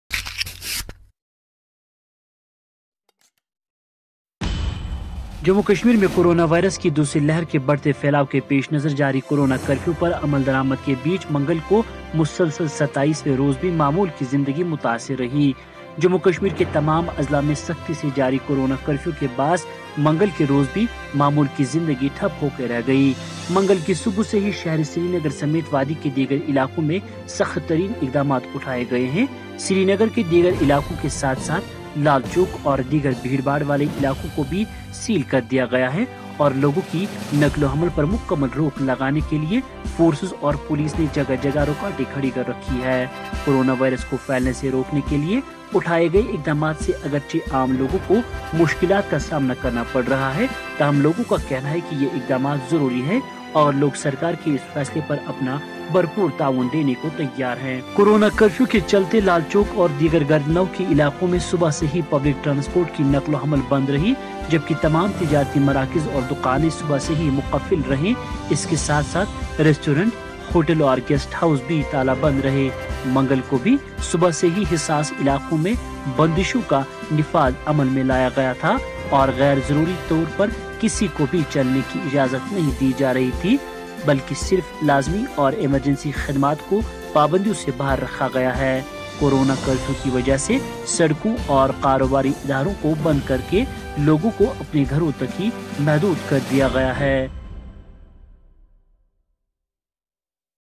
DAY 27: Corona restrictions continues in Parts of J&K
جموں کشمیر میں کرونا وائرس کی دوسری لہر کے بڑھتے پھیلاﺅ کے پیش نظر جاری کورونا کوفیو پر عملدر آمد کے بیچ منگل کو مسلسل 27 ویں روز بھی معمول کی زندگی متاثر رہی ۔جموں کشمیر کے تمام اضلاع میں سختی سے جاری کورونا کوفیو کے باعث منگل کے روز بھی معمول کی زندگی ٹھپ ہو کر رہ گئی ۔منگل کی صبح سے ہی شہر سرینگر سمیت وادی کے دیگر علاقوں میں سخت ترین اقدامات اُٹھائے گئے ہیں ۔سرینگر کے دیگر علاقوں کے ساتھ ساتھ لالچوک اور دیگر بھیڑ بھاڑ والے علاقوں کو بھی سیل کردیا گیا ہے اور لوگوں کی نقل و حمل پر مکمل روک لگانے کےلئے فورسز اور پولیس نے جگہ جگہ رُکاوٹیں کھڑی کررکھی ہے ۔ کروناوائرس کو پھیلنے سے روکنے کےلئے اُٹھائے گئے اقدمات سے اگرچہ عام لوگوں کو مشکلات کاسامنا کرنا پڑرہا ہے تاہم لوگوں کا کہنا ہے کہ یہ اقدامات ضروری ہے اورلوگ سرکارکے اس فیصلے پر اپنا بھر پور تعاون دینے کو تیار ہے ۔کورونا کرفیو کے چلتے لالچوک اور دیگر گردونواح کے علاقوںمیں صبح سے ہی پبلک ٹرانسپورٹ کی نقل وحمل بند رہی جبکہ تمام تجارتی مراکز اور دکانیں صبح سے ہی مقفل رہیں اس کے ساتھ ساتھ رستوران ، ہوٹل اورگیسٹ ہاو س بھی تالہ بند رہے ۔منگل کو بھی صبح سے ہی حساس علاقوں میںبند شوں کا نفاذ عمل میںلایا گیا تھا اور غیر ضروری طور پر کسی کو بھی چلنے کی اجازت نہیں دی جا رہی تھی بلکہ صرف لازمی اور ایمرجنسی خدمات کو پابندیوں سے باہر رکھا گیا تھا ۔ کورونا کرفیو کی وجہ سے سڑکوں اور کاروباری اداروں کو بند کرکے لوگوں کو اپنے گھروں تک ہی محدود کردیا گیا ہے۔ بیورو رپورٹ ویلی آنلائن